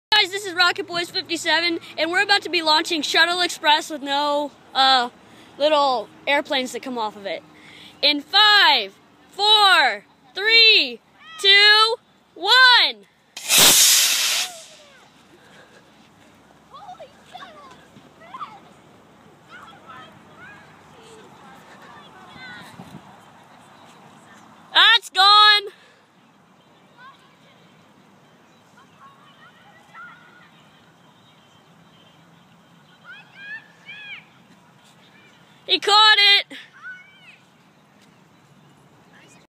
Shuttle express launch
This rocket almost got stuck in a tree we are thankful to have a back!!!